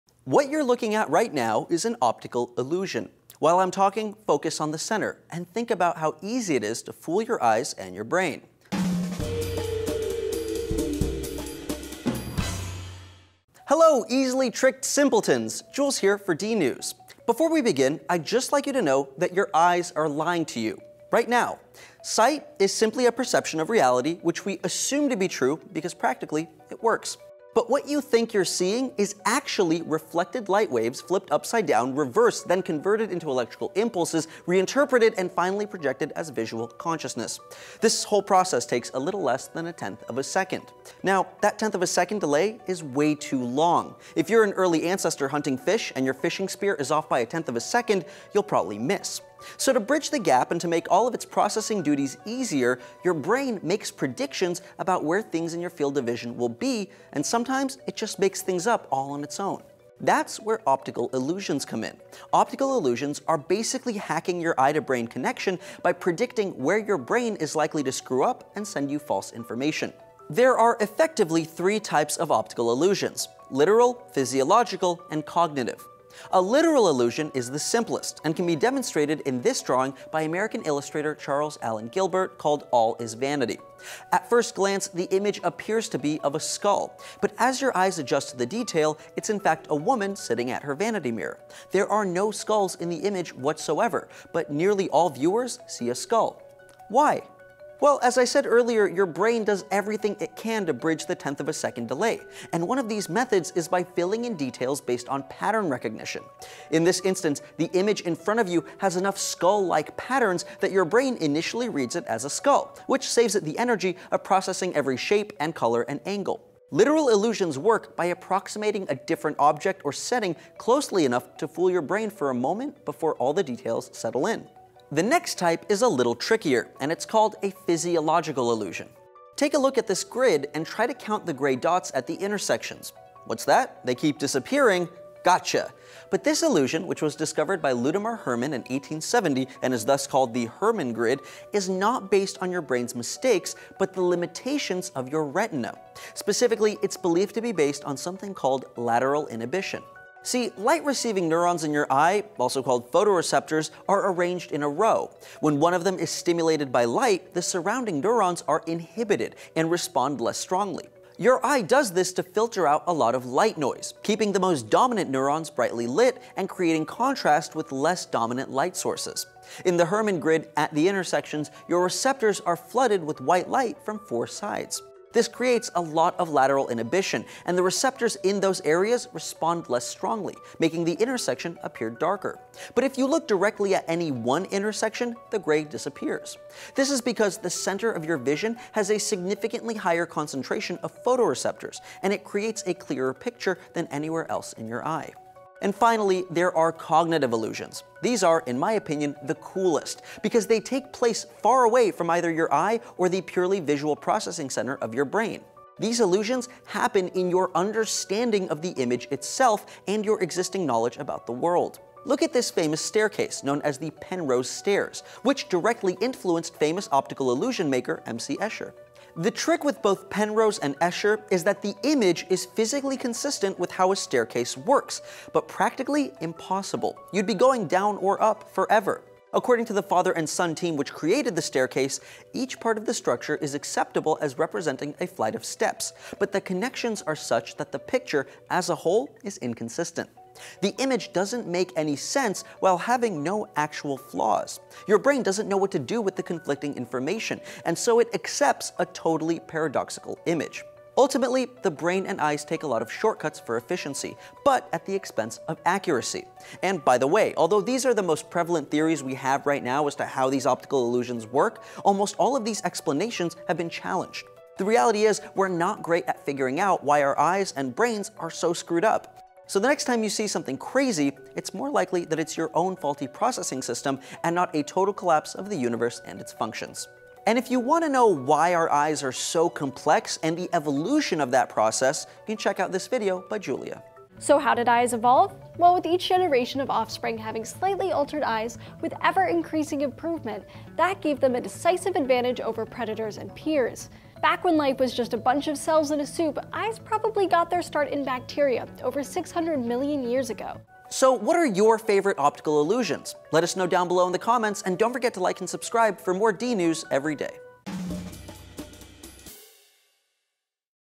How Optical Illusions Trick Your Brain
Things are not at all as they seem... What are the different types of optical illusions and why are our eyes tricking us?

"Visual, or optical, illusions show us that our minds tend to make assumptions about the world - and what you think you see is often not the truth. Throughout history, curious minds have questioned why our eyes are so easily fooled by these simple drawings. Illusions, we have found, can reveal everything from how we process time and space to our experience of consciousness. Scroll down our interactive guide to find out why."

What Are Optical Illusions? - Definition & Types
"In this lesson, we will explore the different types of optical illusions and discuss how they play tricks on our perception. Then you can test your knowledge with a fun quiz."

How Do Optical Illusions Work?
"These are two identical, straight lines. Kind of boring, right? But what happens when you change what's going on around them? All of a sudden, the lines look like they've changed size in relation to each other; they could shrink and grow; they might change to different shades; they may stretch and compress; and they could bend, buckle, or even change directions.  But even though it would seem like the lines themselves are changing, the only thing that's actually bending or buckling is your mind -- the truly mesmerizing part of optical illusions."

Special thanks to Jules Suzdaltsev for hosting DNews!